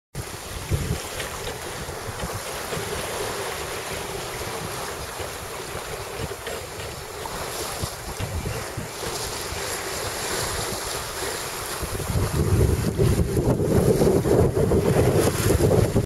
Boulogne sur Mer - Nieuwpoort
First posibility to sail after using the engine for about 6h30